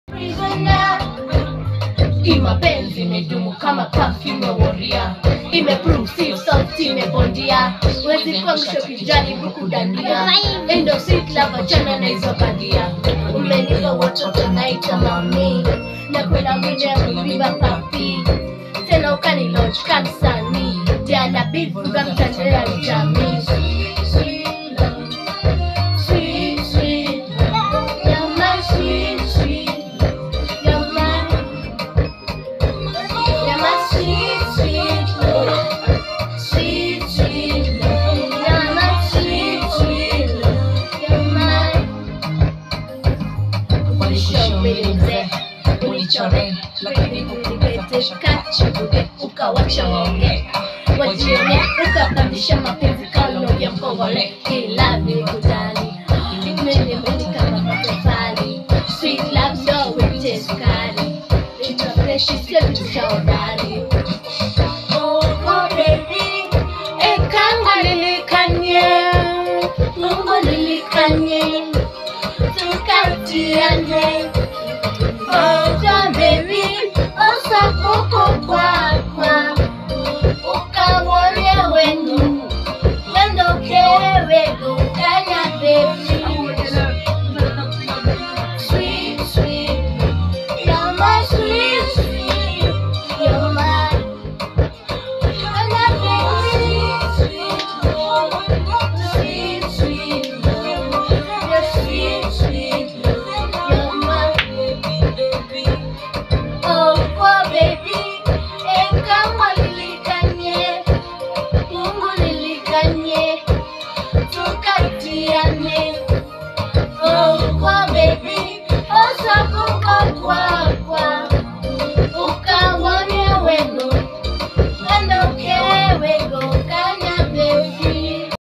Love wins ..........
The love this young girl has for Diana B banange. Diana B should watch this